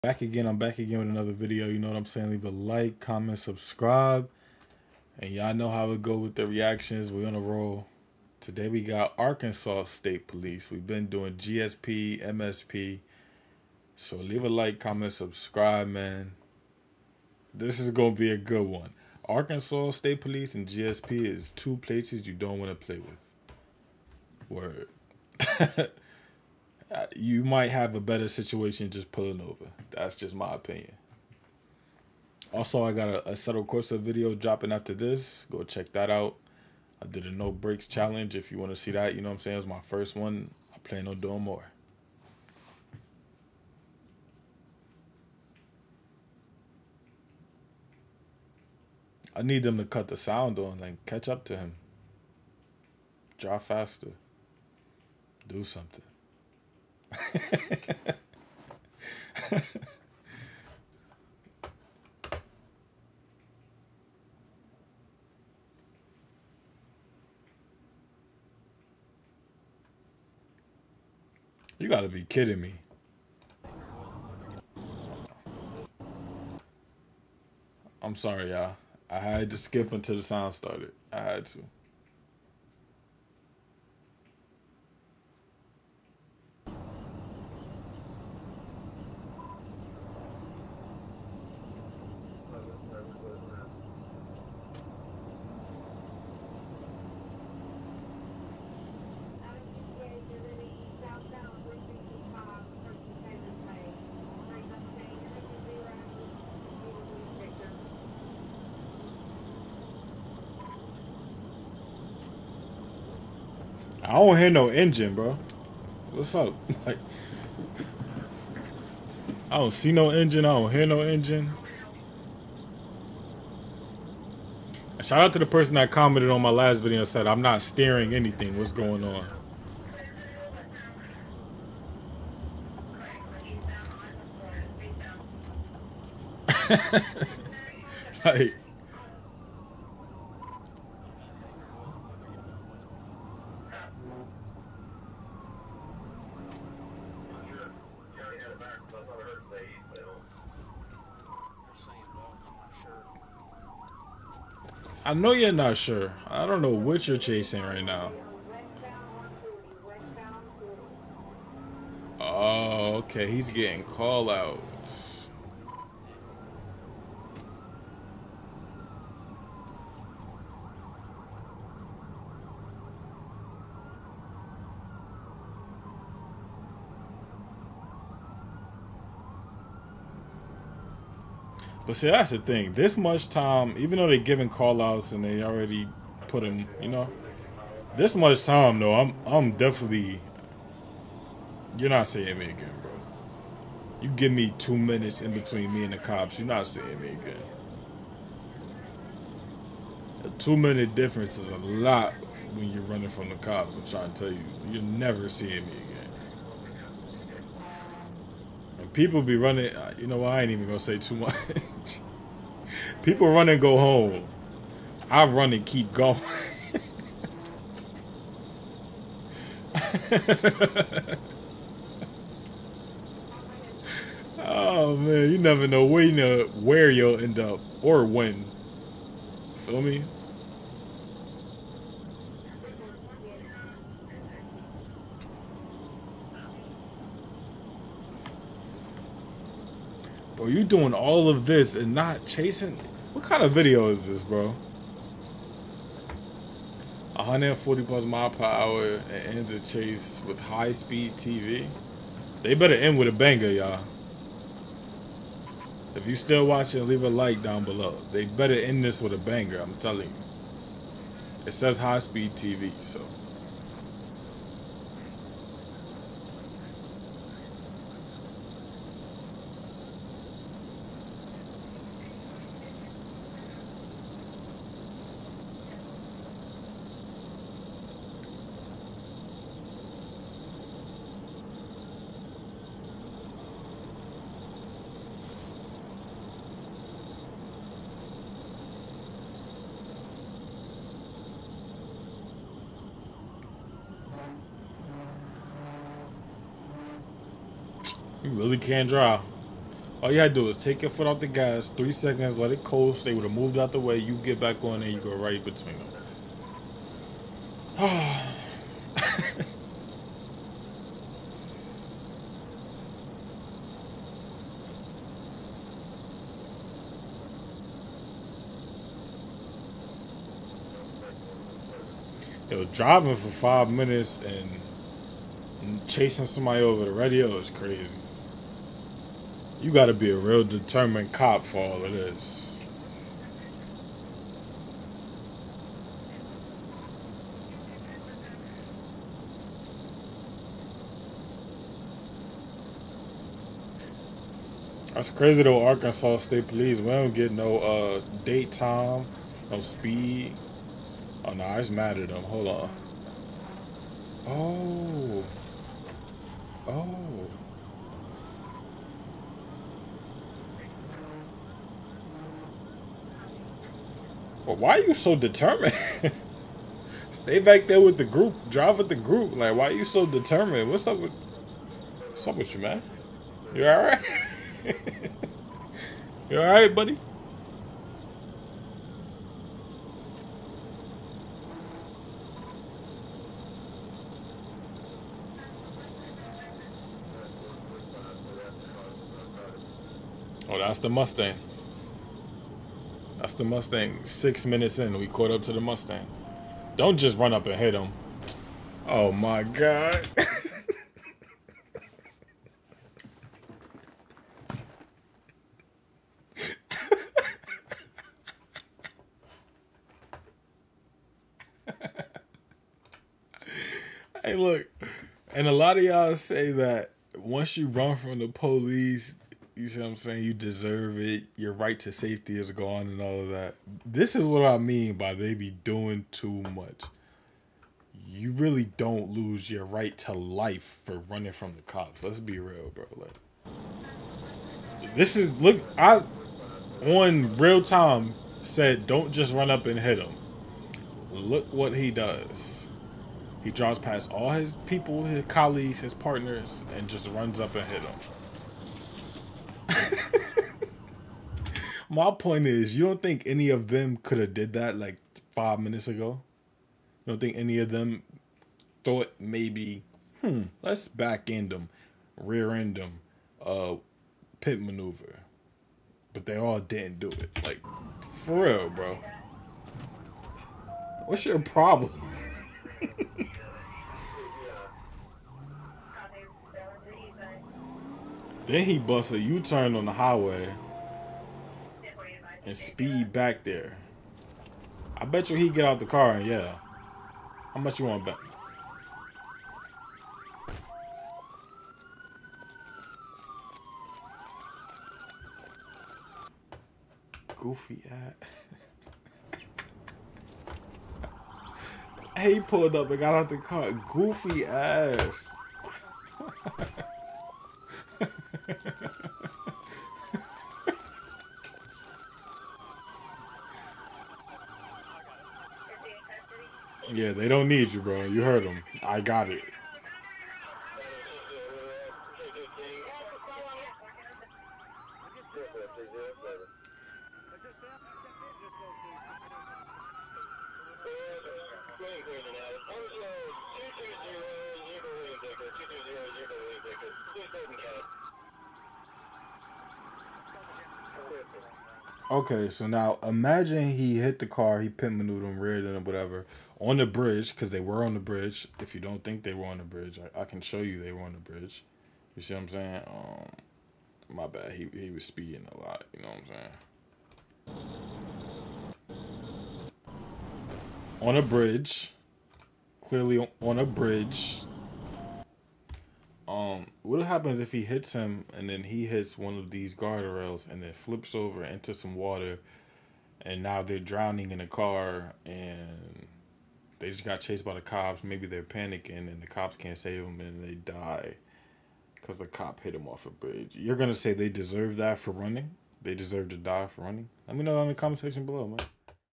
Ford Mustang greeted by the Arkansas State Police PIT Maneuver at 103 MPH REACTION
Twitch.TV/Grammyyb